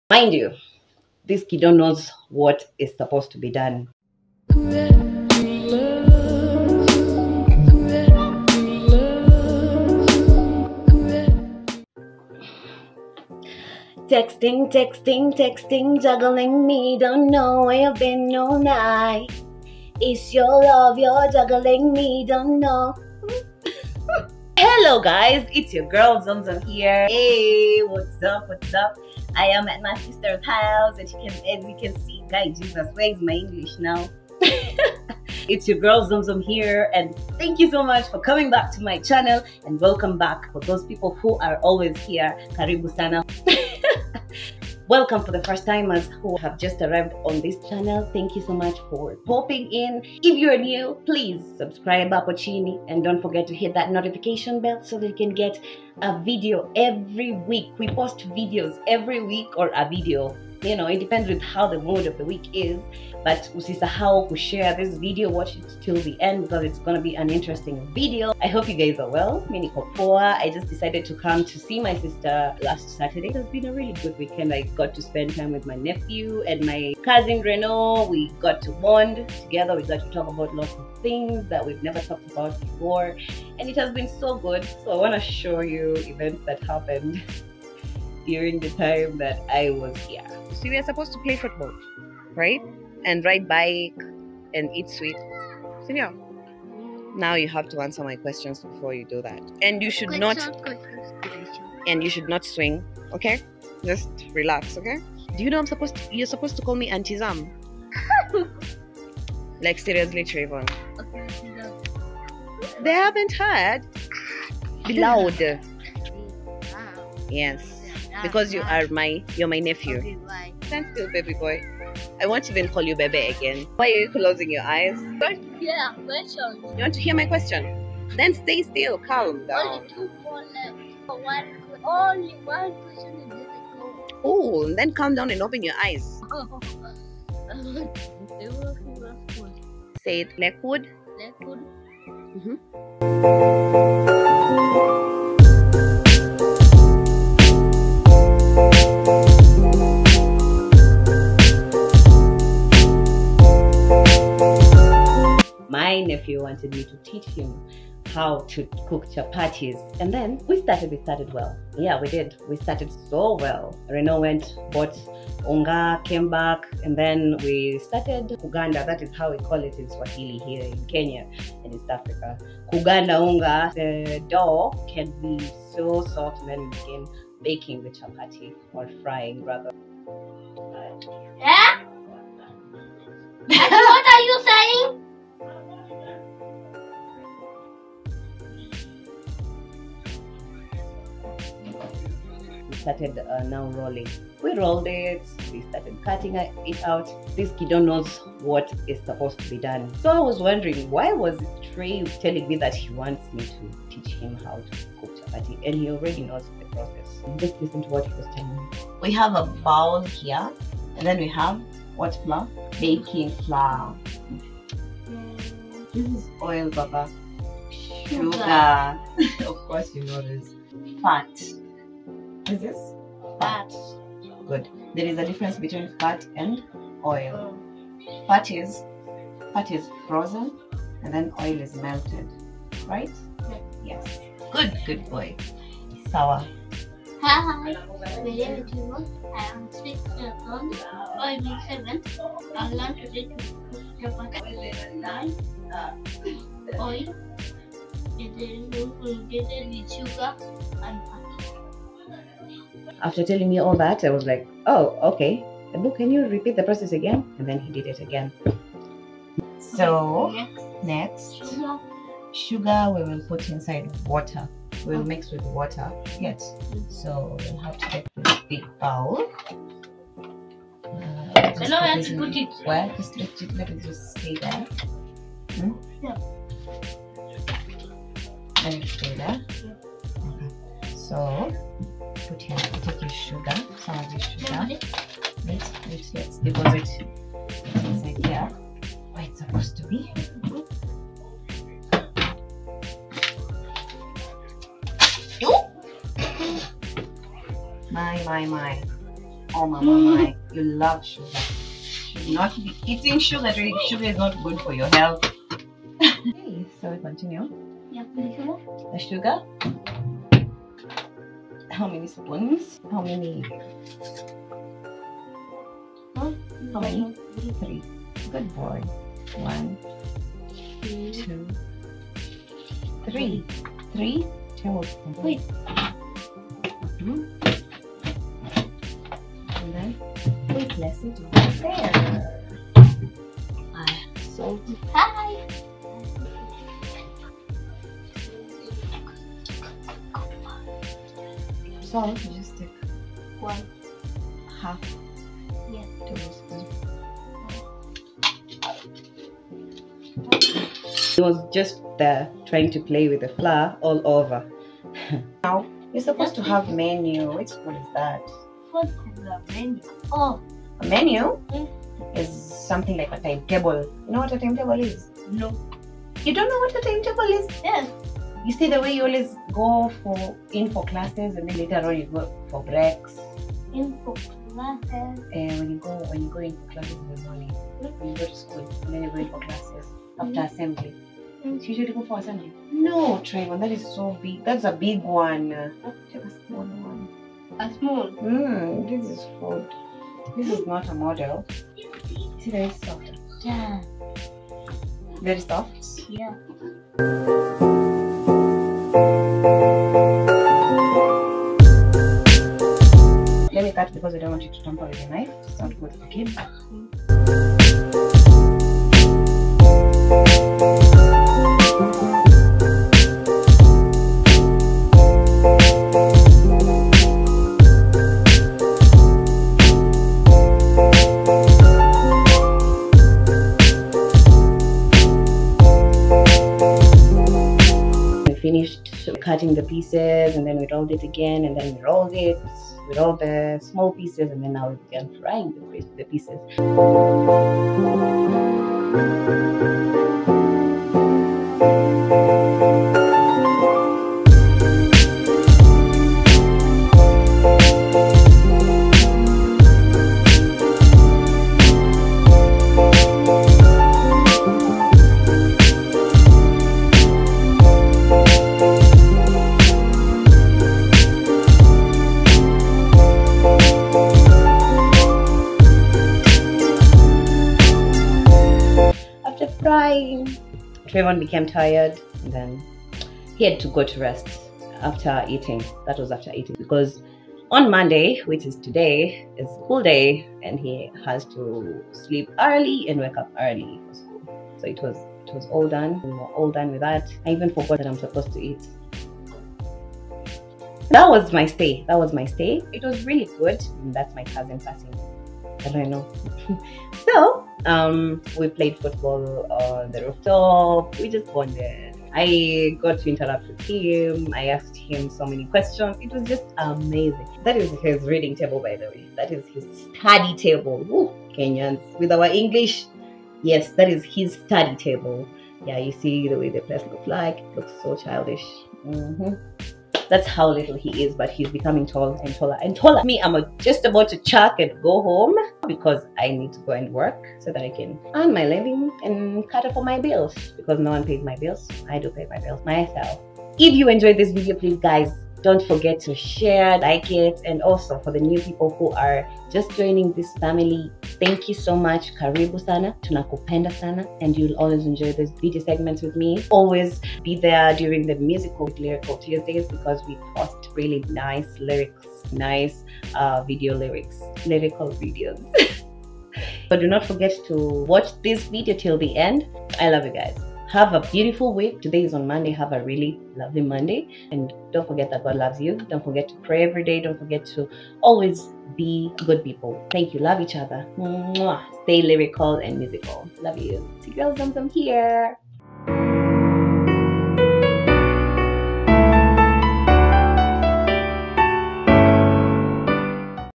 A Day in Life With My Nephew Trayvon || Your Girl Zamzam
Spent sometime with my sister, nephew and cousin during the past weekend and it was a really cool experience as I got to restore the lost bond I had had with Trayvon when he was still tiny.